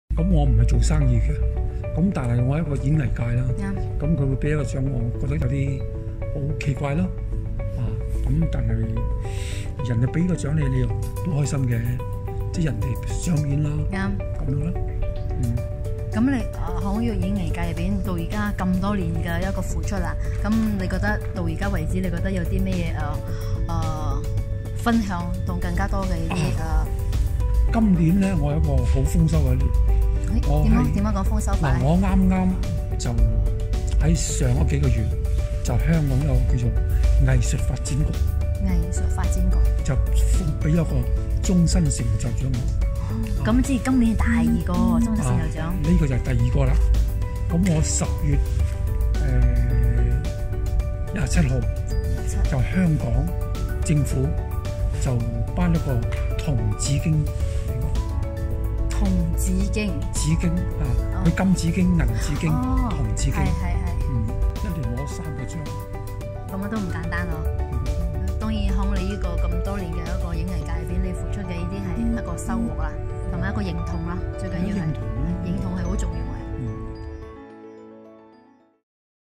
[Exclusive Interview] 罗家英博士 Dr. Law Kar Ying
it's our pleasure to have a short exclusive interview with Hong Kong famous actor Dr. Law Kar Ying 罗家英博士